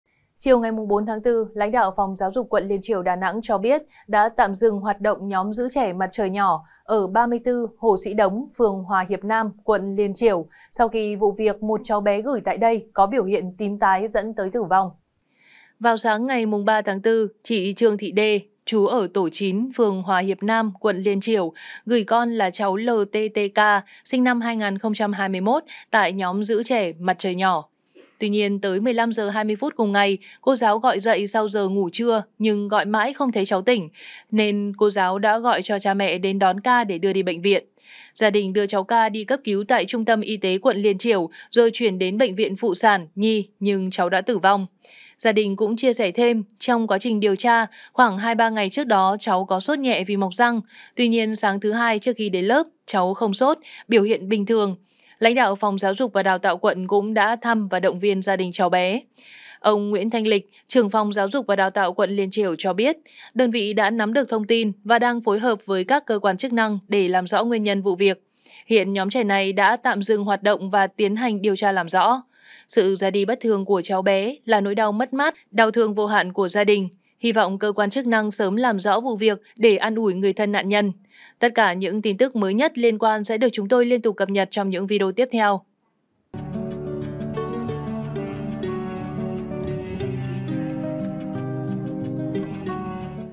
Lý Do Đình Chỉ Cơ Sở Trông Trẻ Xảy Ra Vụ Cháu Bé Hai Tuổi Tử Vong Bất Thường Ở Đà Nẵng | SKĐS
SKĐS | Sáng ngày 3/4, sau khi đưa cháu bé hai tuổi tới nhóm gửi trông trẻ Mặt Trời Nhỏ, cháu bé có biểu hiện tím tái. Sau khi được người nhà đưa vào bệnh viện cấp cứu cháu đã tử vong. Nhóm gửi trông trẻ buộc tạm dừng hoạt động để lực lượng công an tiến hành điều tra làm rõ.

Mời quý vị và các bạn xem thêm: Hé Lộ Nguyên Nhân Ban Đầu Vụ Nổ Như Bom Ở Nghệ An Khiến 6 Người Thương Vong